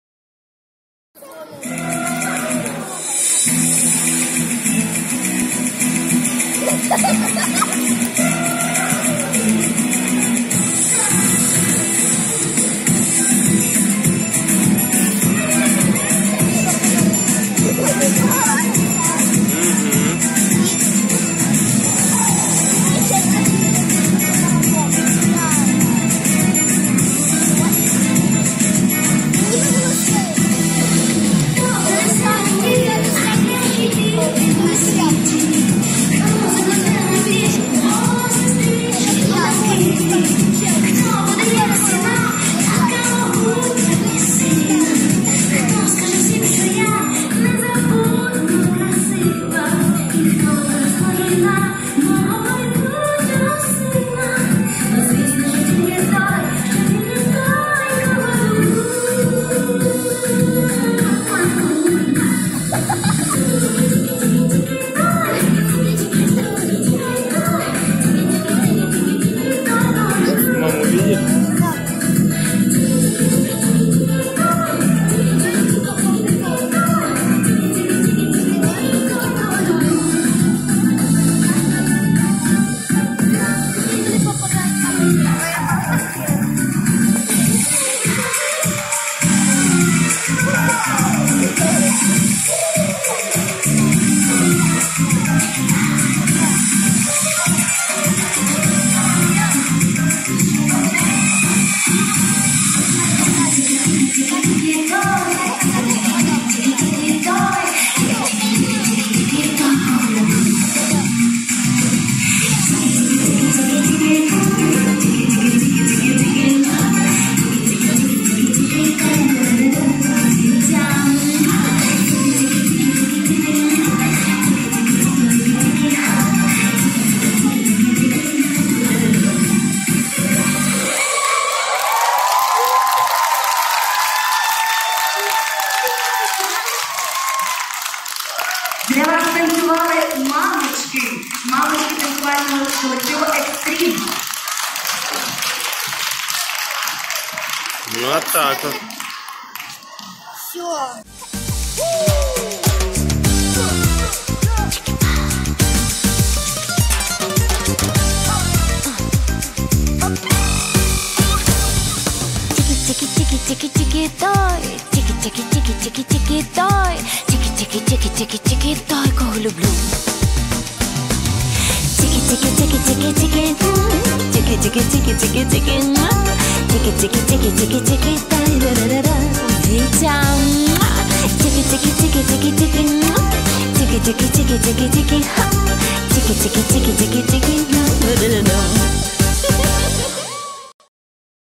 Танец мамочек на фестивале Танца в Ивано-Франковске
Огромное спасибо нашему тренеру за возможность не только привезти деток на фестиваль, но и  самим мамам принять участие в мероприятии) Это было здорово!!!